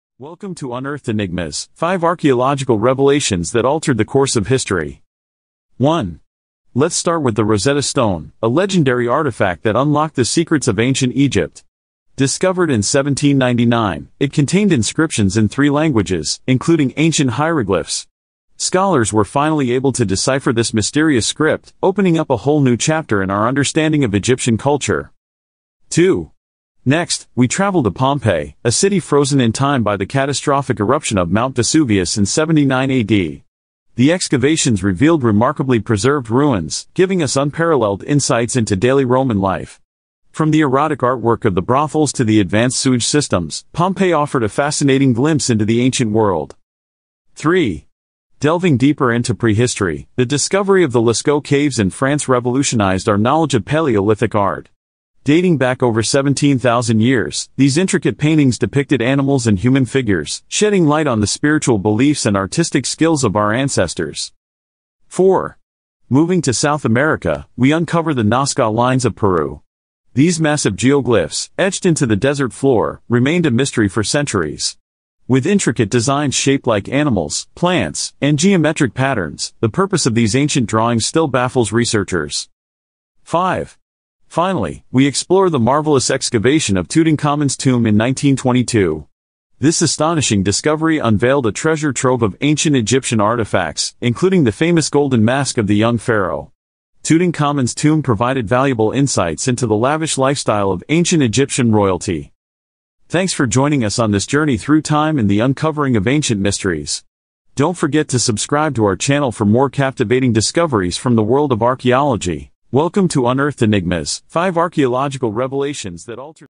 Unearthed Enigmas: 5 Archaeological Revelations That Altered the Course of History
In this captivating video, we delve into the hidden depths of history to unveil intriguing ancient mysteries. Join us as we explore 5 groundbreaking archaeological discoveries that have forever transformed our understanding of the past.

Prepare to be amazed as we unravel the enigmatic tales behind these extraordinary finds, shedding light on civilizations long forgotten. From unearthing lost artifacts to deciphering ancient inscriptions, each revelation offers a unique glimpse into the past and reshapes the course of humanity's narrative.

Our expert team of archaeologists and historians guide you through the unrivaled significance of each discovery, drawing connections to the impact they had on shaping our world today. Witness firsthand the awe-inspiring relics and astounding revelations that have puzzled and fascinated researchers for centuries.

By combining meticulous research, cutting-edge technology, and the tenacity of curious minds, these archaeological breakthroughs challenge long-held beliefs and rewrite the annals of history. From ancient ruins to burial sites, prepare to embark on a journey that unearths the secrets locked away beneath the sands of time.

Whether you're a history enthusiast, a lover of mysteries, or simply intrigued by the monumental discoveries that have shaped our world, this video is a must-watch. Join us as we part the veil of uncertainty, bringing you face-to-face with the wonders that have forever altered our perception of the past.